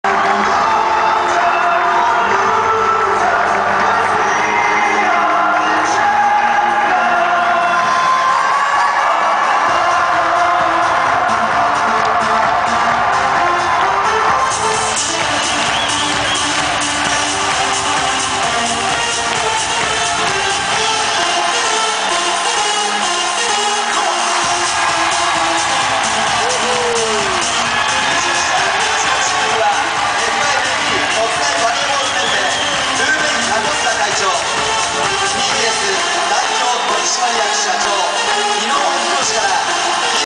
We are The Champions!
Brazil on the podium to receive GOLD medal.